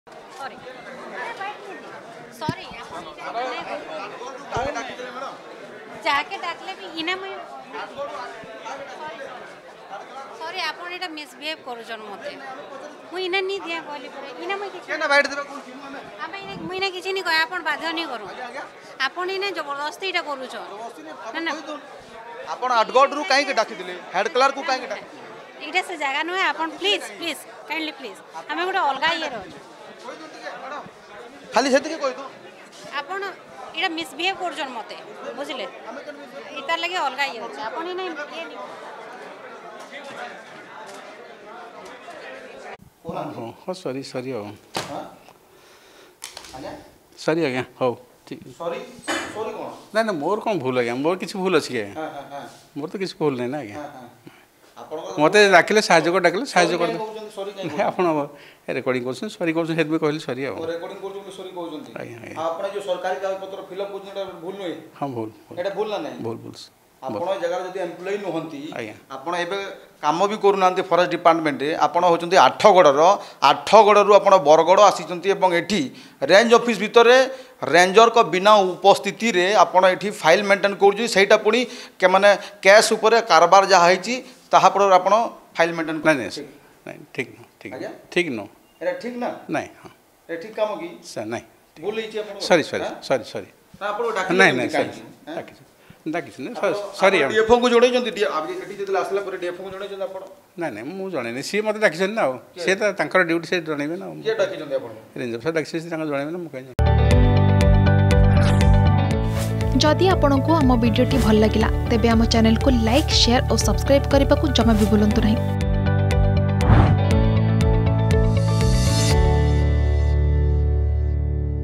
ରେଞ୍ଜରଙ୍କ ଚୌକିରେ ଇଏ କିଏ ? | Forest Range Officer | Bargarh
ଅର୍ଗସ କ୍ୟାମେରା ଦେଖି ଫେରାର ମାରିଲେ ରେଞ୍ଜ ଅଫିସର । ପୁଣି ବିବାଦରେ ବରଗଡ଼ ବନ ବିଭାଗ ।